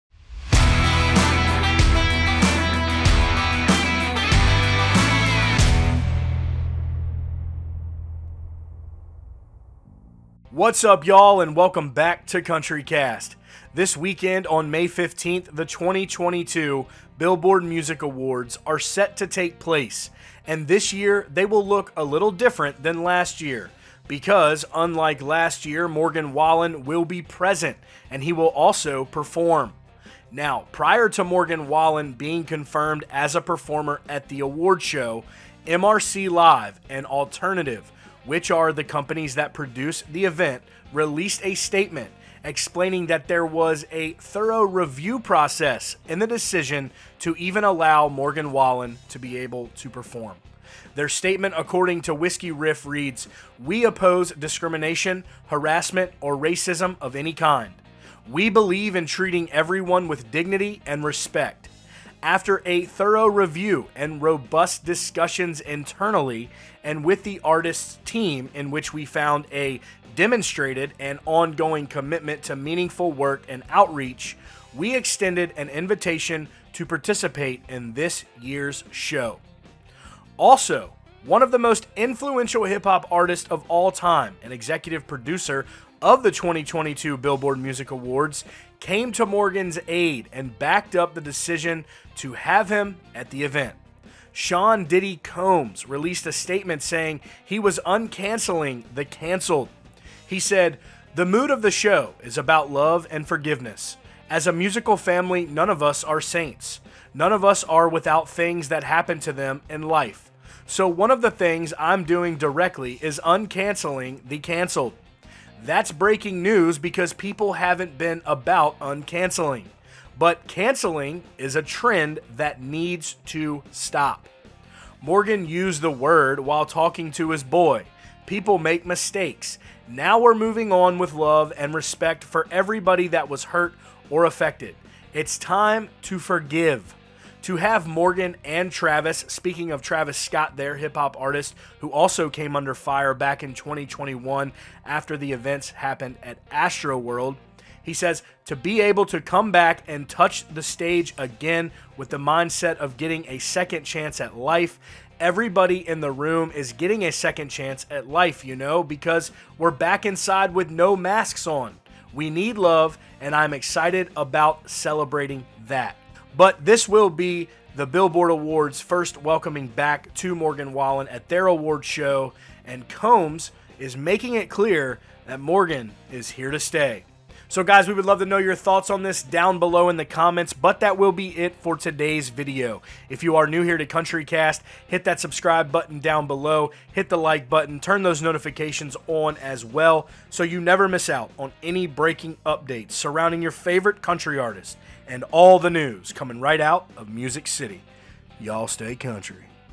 Sean ‘Diddy’ Combs Supports Morgan Wallen: Cancelling Needs To Stop!
Sean "Diddy" Combs, one of the most influential hip-hop artosts of all time recently defended Morgan Wallen performing at the upcoming 2022 Billboard Music Awards. Morgan Wallen was banned in 2021, but in 2022 Executive Producer Sean "Diddy" Combs has other plans.